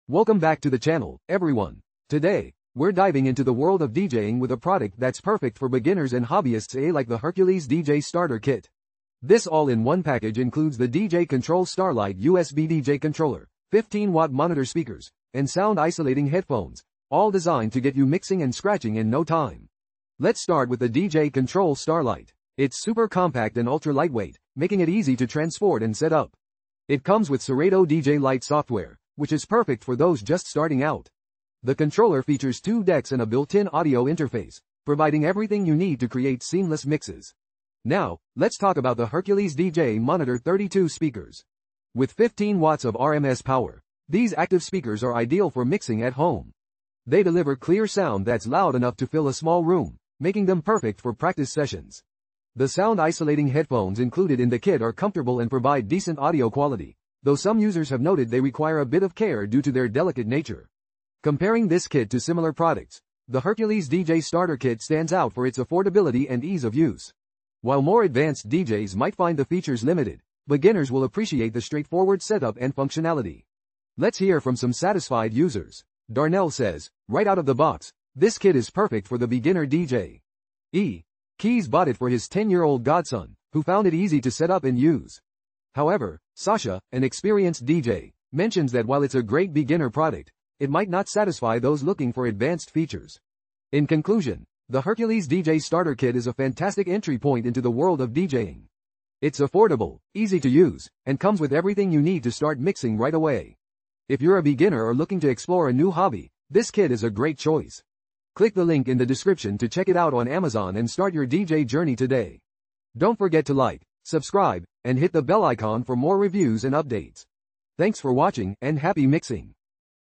How to Start DJing: Hercules DJ Starter Kit Review & Setup
See product in Amazon

🎧🎶 Dive into the world of DJing with the Hercules DJ Starter Kit! This all-in-one DJ kit is your perfect gateway to mixing and scratching like a pro. Whether you're a beginner or looking to reignite your passion for music, this kit has everything you need to get started. Featuring the DJControl Starlight USB DJ Controller, 15-Watt Monitor Speakers, and Sound-Isolating Headphones, it's designed for easy setup and portability. Ideal for small spaces, this kit is perfect for anyone eager to explore the art of DJing.

The Hercules DJ Starter Kit is equipped with the DJControl Starlight, a super-compact and ultra-lightweight controller that pairs seamlessly with Serato DJ Lite software. This combination offers a user-friendly interface, making it easy for beginners to dive into the world of DJing. The 2-deck controller with a built-in audio interface ensures you have all the essential features for mixing and scratching. The included Hercules DJ Monitor 32 speakers deliver 15 watts of RMS power, providing clear and powerful sound for your home mixing sessions.

Customer reviews highlight the kit's suitability for beginners and its ease of use. Many users appreciate the quality of the speakers and the comfort of the headphones, although some note that the headphones require careful handling. The DJControl Starlight's jog wheels offer a vinyl-like experience, which is a hit among users familiar with traditional DJ setups. However, experienced DJs may find the Serato DJ Lite software limiting, as it lacks advanced features like recording and sound effects.

Overall, the Hercules DJ Starter Kit is a fantastic choice for those new to DJing or looking for a budget-friendly option to explore this exciting hobby. With an average rating of 4.1 out of 5 stars from 453 ratings, it's clear that this kit is a popular choice among aspiring DJs.

0:00 Welcome Back To The Channel
0:02 Introduction to the Hercules DJ Starter Kit
0:09 Overview of the DJ Control Starlite
0:31 Features of the Hercules DJ Monitor 32 Speakers
0:52 User Reviews and Comparisons
1:33 Conclusion and Recommendations
1:48 Call to Action and Closing Remarks